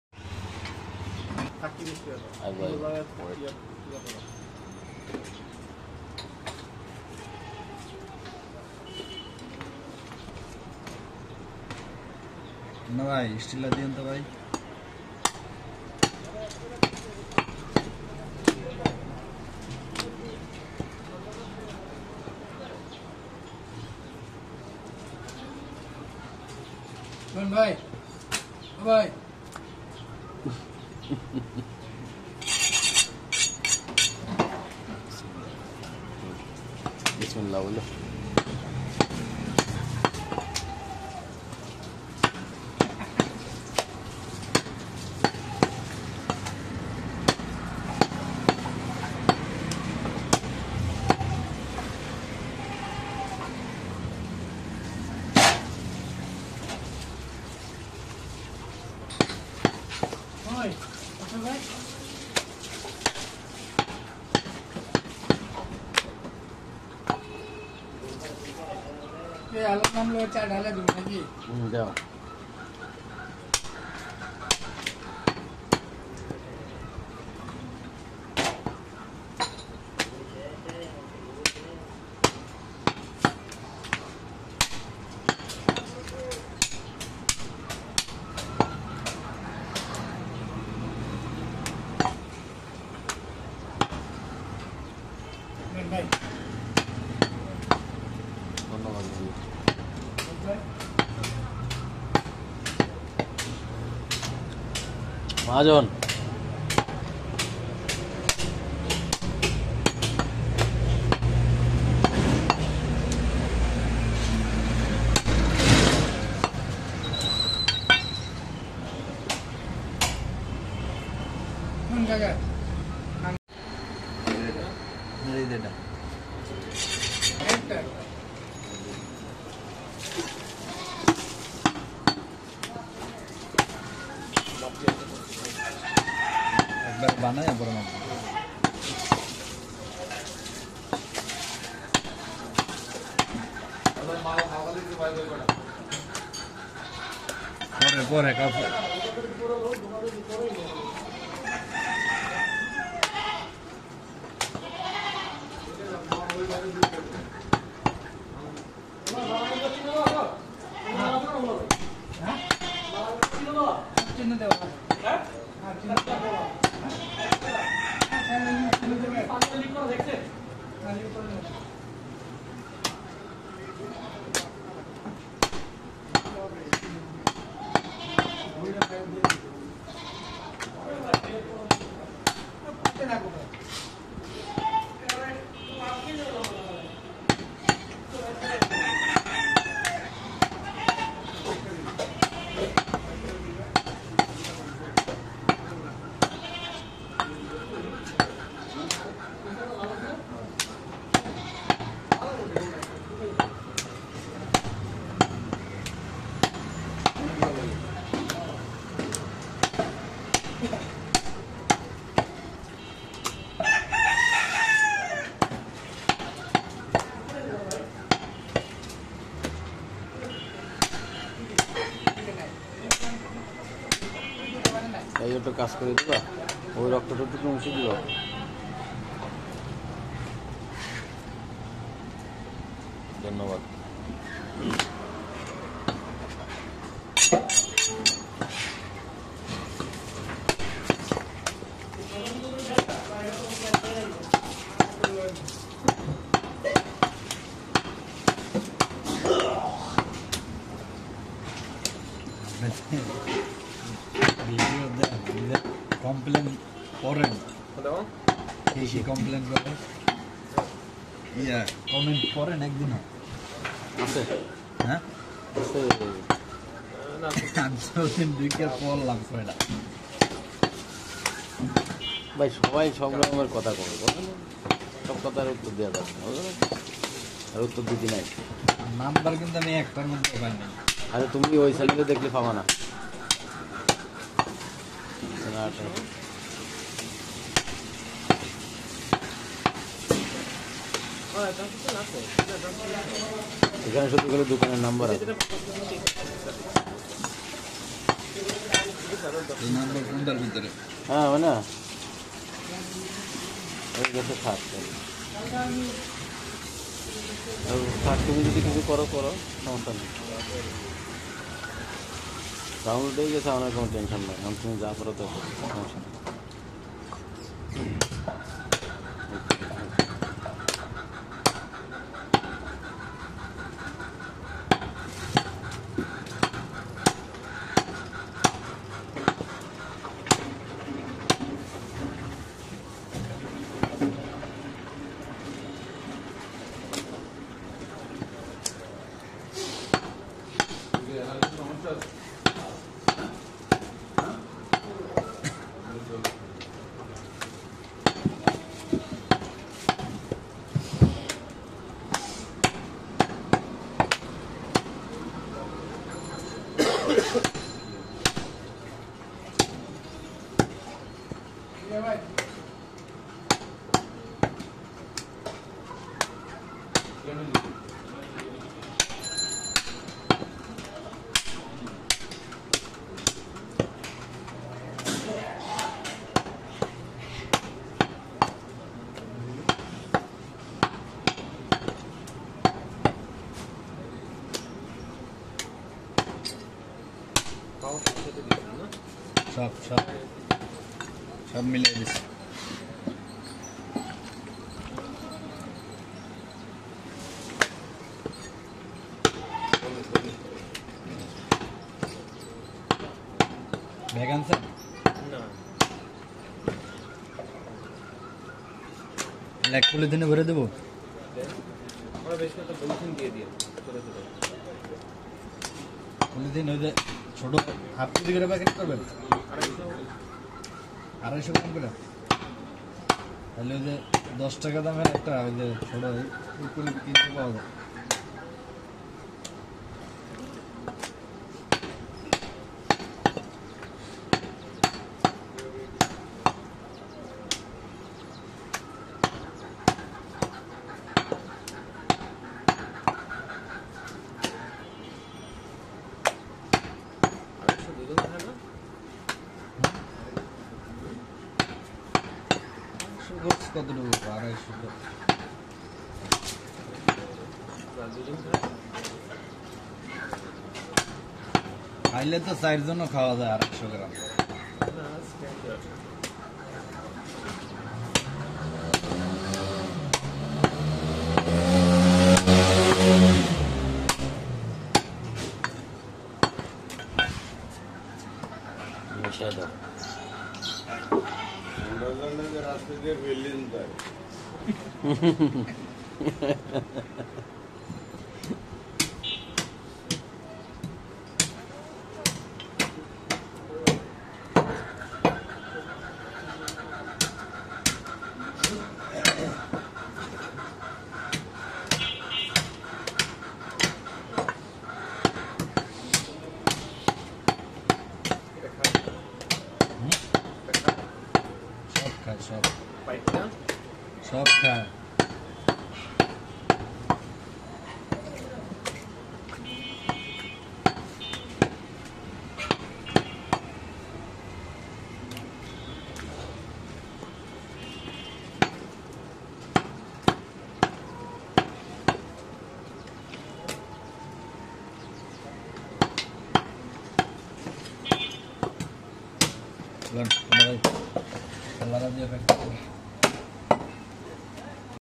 Big Goat Legs Meat Cutting | Number One Mutton Cutting & Selling Shop | Goat Meat Cutting Skills
Harun's Khasi Meat Shop - Mirpur-06, Dhaka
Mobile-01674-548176